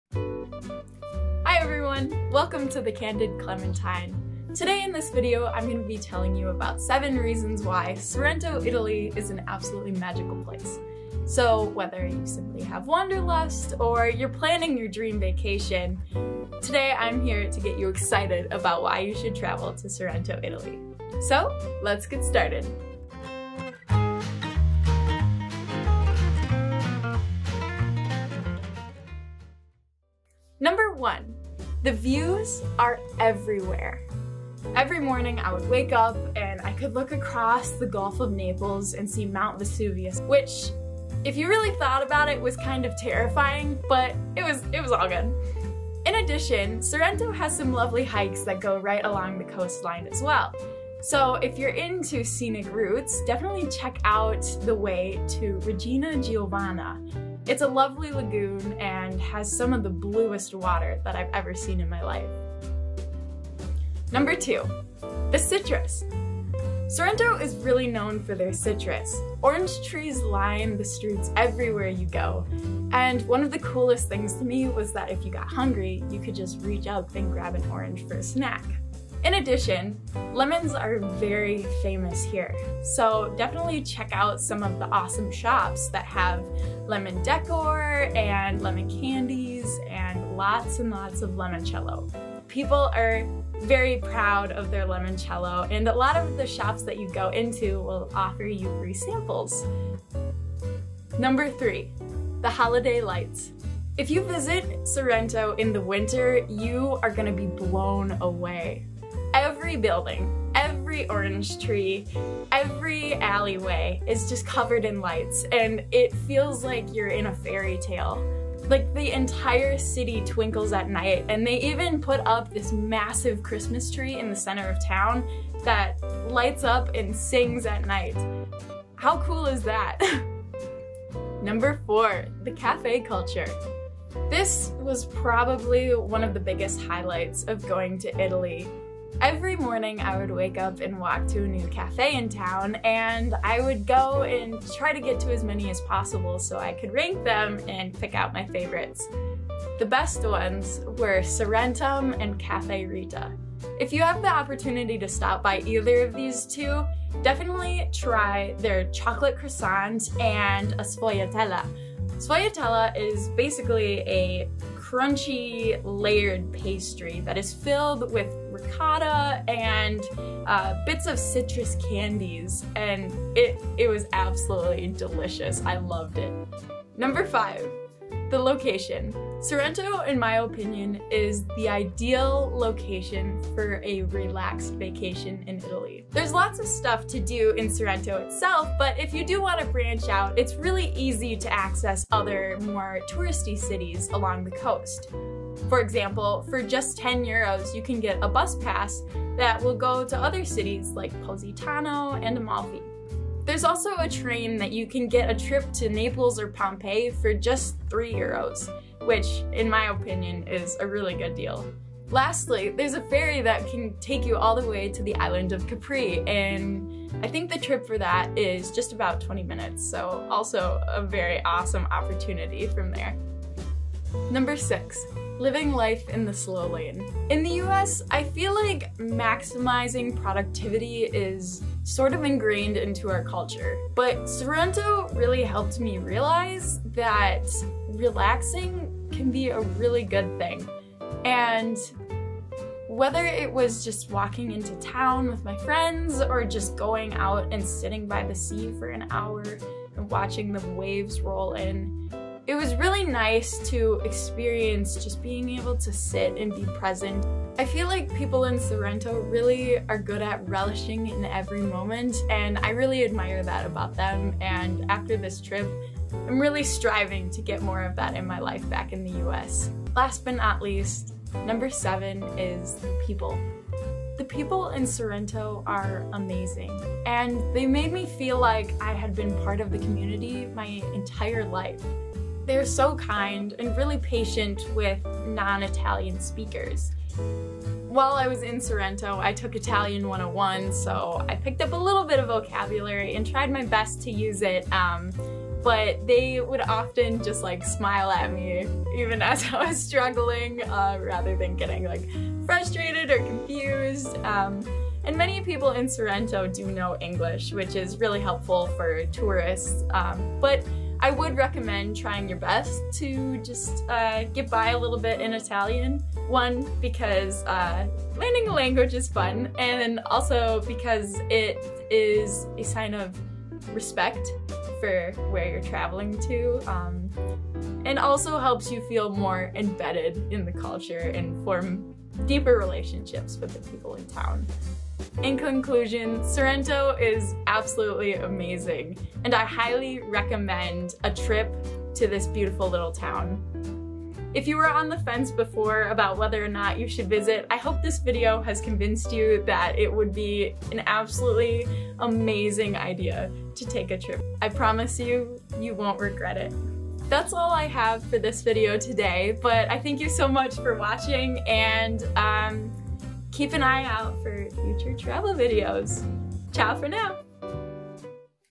Adventures in Sorrento, Italy: Why you should visit the Amalfi Coast
I created this video after my study abroad in Italy. I fell in love with Sorrento while I was there and so I wanted to showcase some of its magic for you with this video.

Side note: I finished this video about a year ago, but never got around to uploading it. I recently stumbled across it again and thought, welp, here goes nothing!